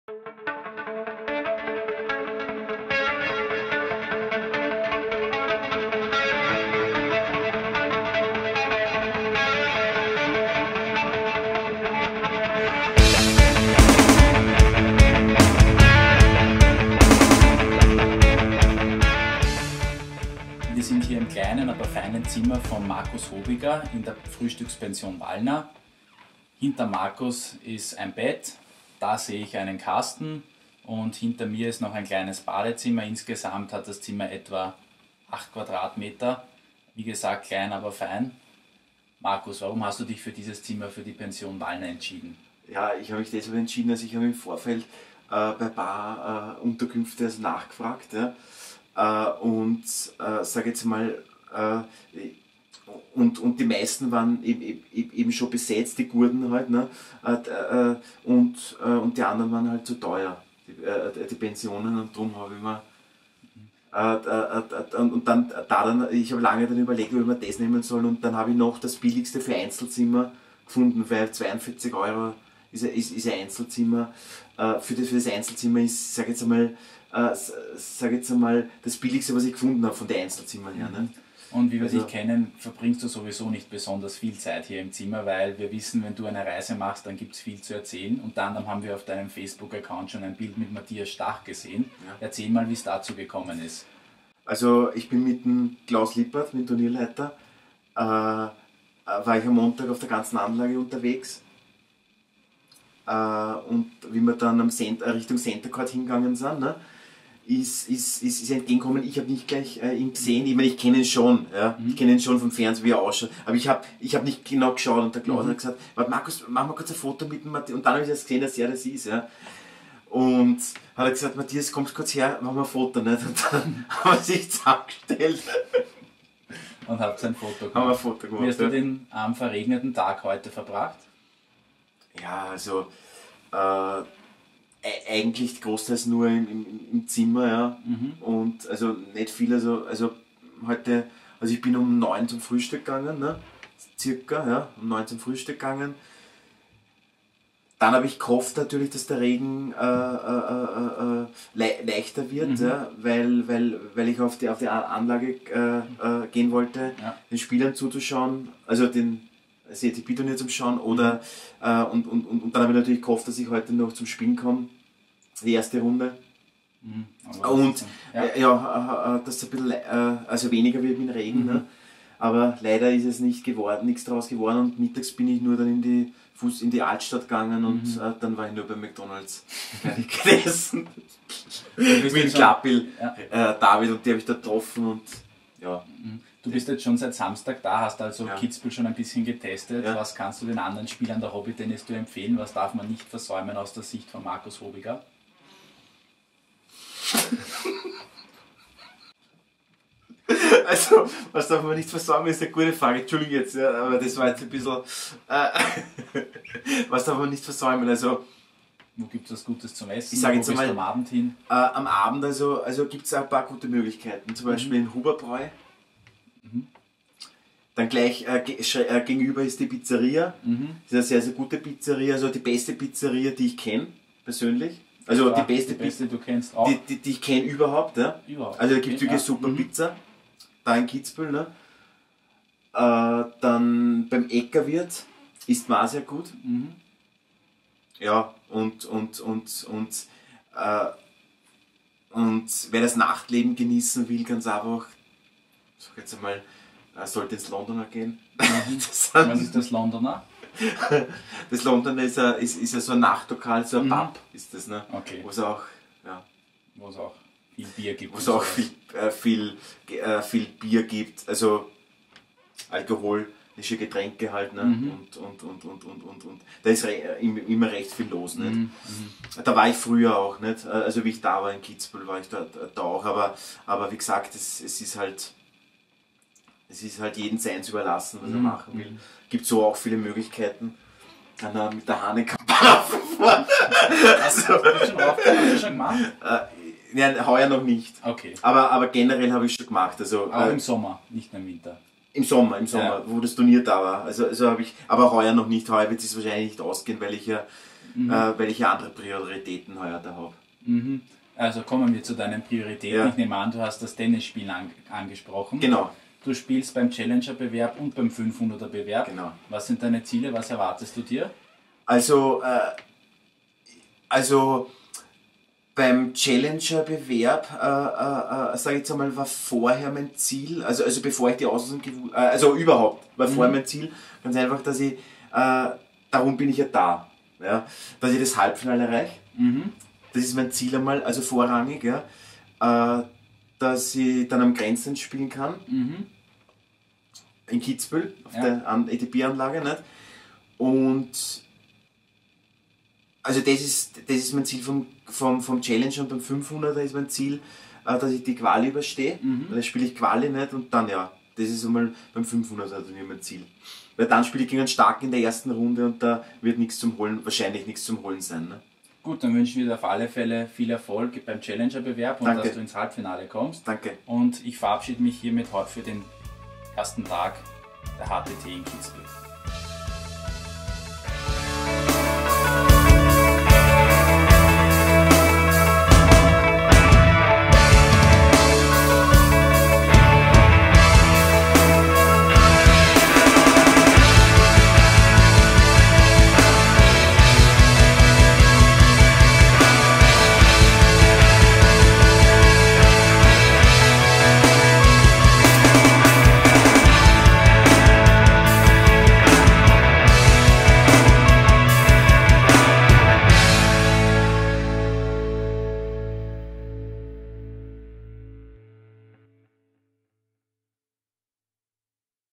Kitzbühel Home Stories 2014 - Markus Hobiger
Zu Gast im Quatier bei Markus Hobiger im Rahmen der Babolat Kitzbühel Trophy 2014 der hobbyTennisTour.